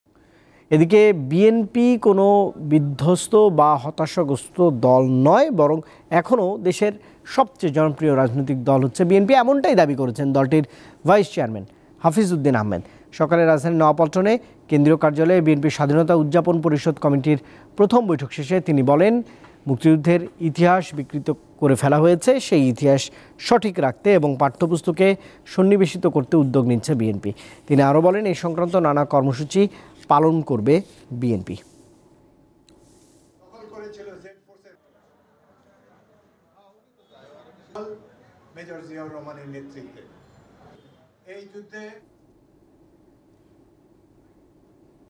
বিএনপি কোনো বিধ্বস্ত বা হতাশাগ্রস্ত দল নয়: মেজর হাফিজ | Major Hafeez | BNP | Deepto News
Address:
Deepto TV
7/A/GA Tejgaon Industrial Area,